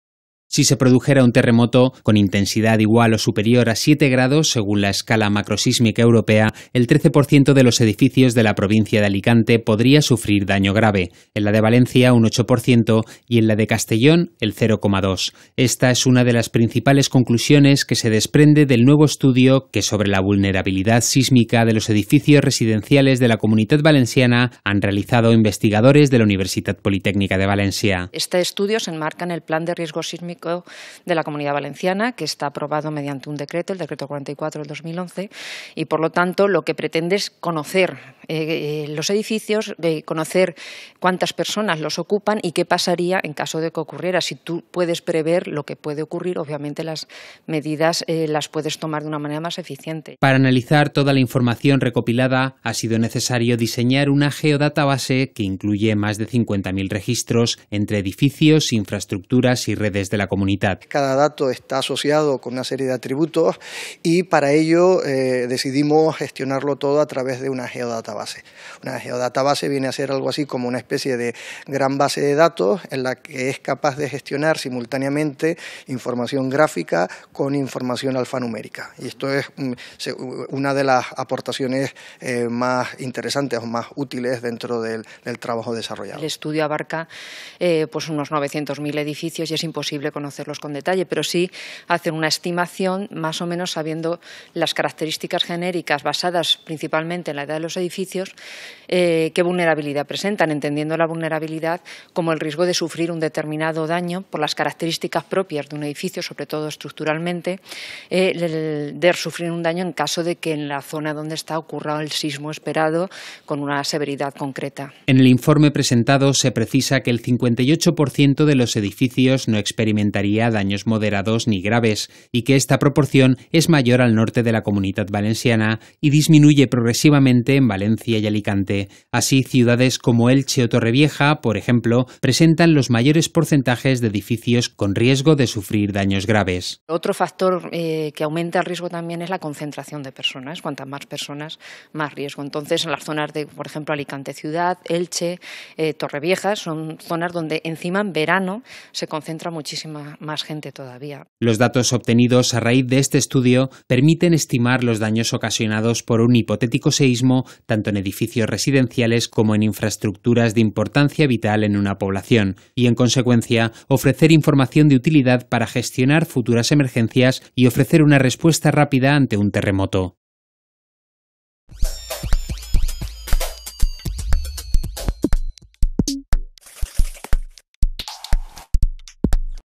Estudio vulnerabilidad sísmica en la Comunitat Valenciana - Noticia @UPVTV, 12-01-2016
Si se produjera un terremoto – con intensidad igual o superior a VII grados según la Escala Macrosísmica Europea – el 13 por ciento de los edificios de la provincia de Alicante podría sufrir daño grave; en la de Valencia, un 8 por ciento y en la de Castellón, el 0,2. Esta es una de las principales conclusiones que se desprende del nuevo estudio que sobre la vulnerabilidad sísmica de los edificios residenciales de la Comunitat Valenciana han realizado investigadores de la Universitat Politècnica de València.

Para analizar toda la información recopilada ha sido necesario diseñar una geodatabase que incluye más de 50.000 registros; entre edificios, infraestructuras y redes de la Comunitat.

En el informe presentado se precisa que el 58 por ciento de los edificios no experimentaría daños moderados ni graves, y que esta proporción es mayor al norte de la Comunitat Valenciana y disminuye, progresivamente, en Valencia y Alicante. Así, ciudades como Elche o Torrevieja; por ejemplo, presentan los mayores porcentajes de edificios con riesgo de sufrir daños graves.

Los datos obtenidos a raíz de este estudio permiten estimar los daños ocasionados por un hipotético seísmo, tanto en edificios residenciales como en infraestructuras de importancia vital en una población. Y, en consecuencia, ofrecer información de utilidad para gestionar futuras emergencias y ofrecer una respuesta rápida ante un terremoto.